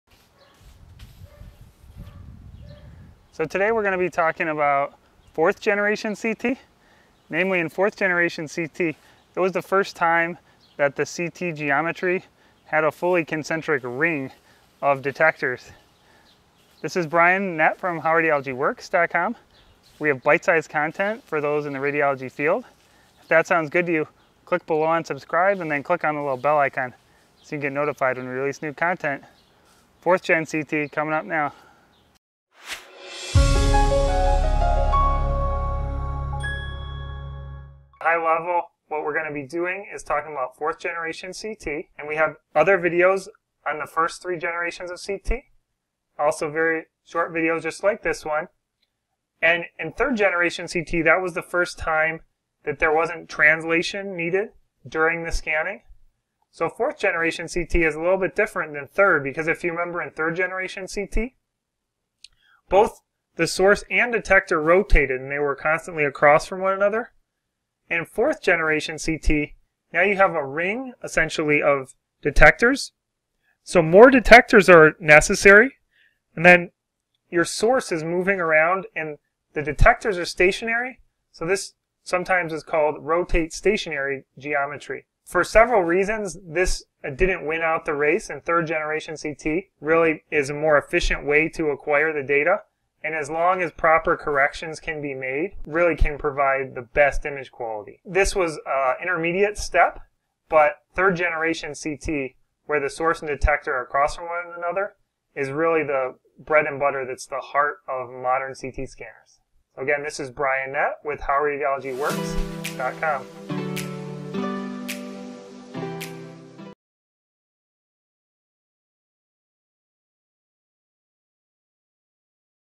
Fourth Generation CT Radiologic Technologists
For a detailed description please see our blog post on CT generations

chapters:
00:00 intro
00:39 4th Gen CT

First Generation CT:
This is the first time that CT was performed and is the basis for the Nobel Prize in Medicine.

The very first CT scans were performed on a first generation geometry on a CT benchtop. In the benchtop systems patients are not imaged but rather an object to be imaged is placed on a stage that can rotate (i.e. like a slow and well calibrated record player).

Then in order to image patients (rather than biological samples) a rotating gantry is needed so that the patient can lie on an imaging table and the x-ray source and detector will rotate around the patient.

Both of the benchtop and first generation rotating CT systems share a common configuration that is referred to as a translate/rotate acquisition.

In first generation CT scanners, there was one X-ray source and one X-ray detector. So, in order to acquire an axial image of the patient, one ray would go through body of patient and be measured using a single detector. The x-ray source and the detector moved together to collect the data. In order to reconstruction one slice the x-ray source would have to translate many times for each view. Then the source and tube were rotated with respect to the patient (or another object being imaged).

This process was very time-consuming and scanners were slow. Such scanners with only one source and one detector proved that there was tremendous value in CT but they could be very slow taking ½ hr for an average acquisition of several slices.